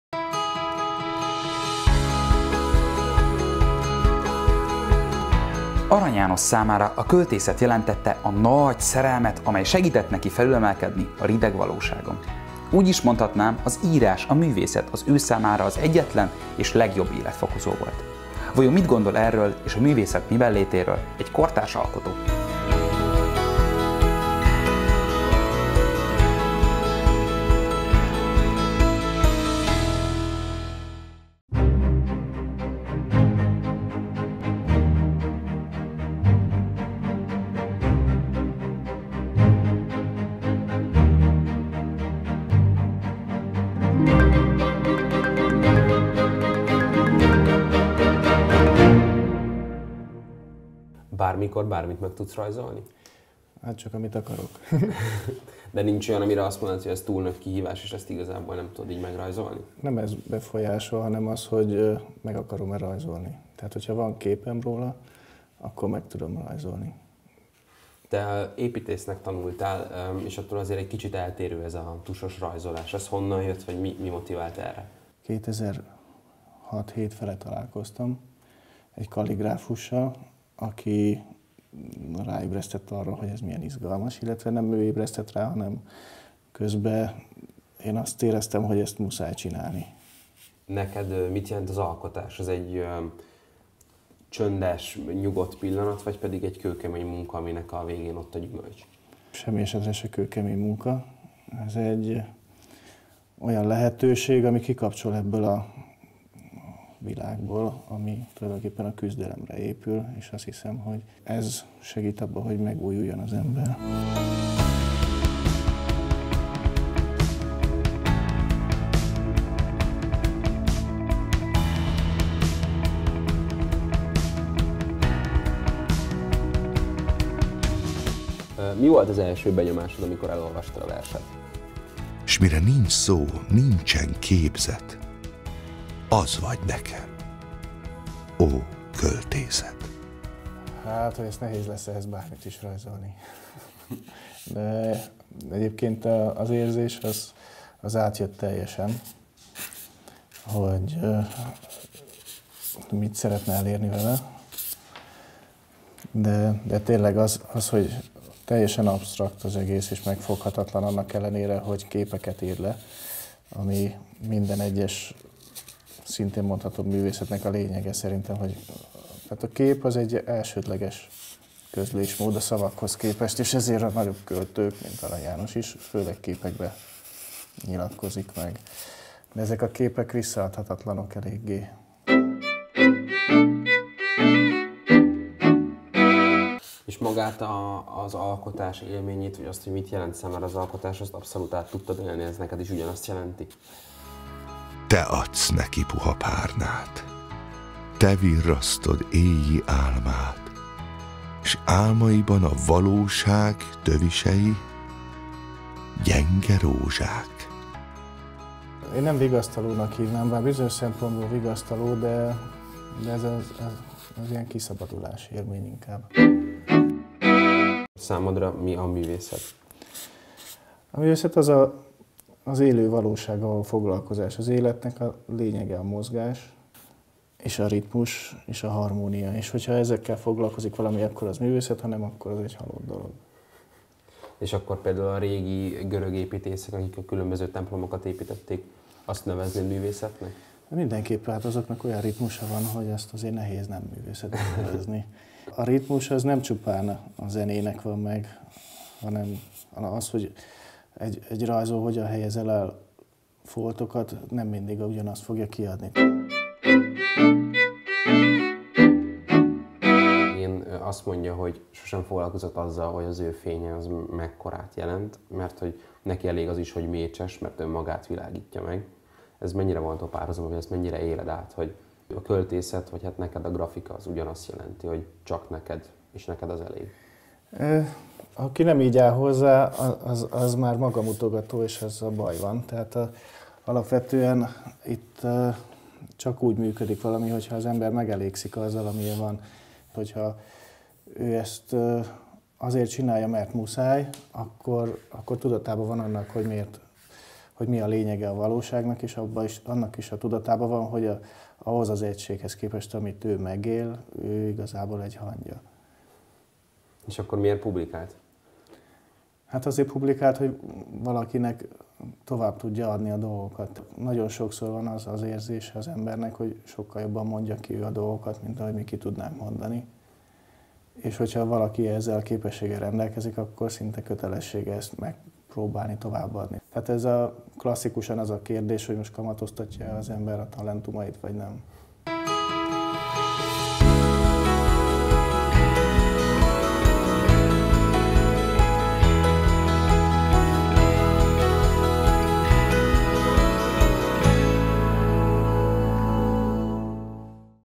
Gondolta a fene! – Arany János: A vigasztaló
Azok az emberi érzések, szituációk, jelenségek, amelyeket Arany János megélt és megírt, ma is ugyanúgy jelen vannak, foglalkoztatják a kor emberét. A Gondolta a fene! egy rendhagyó sorozat, amelyben nem csak költők, írók beszélnek Arany Jánossal kapcsolatos gondolataikról, hanem
filmrendezők, színészek, dalszerzők, papok, pszichológusok is, akiknek egytől-egyig személyes élményt jelent egy-egy Arany-vers, ballada, a költő életművének ismerete.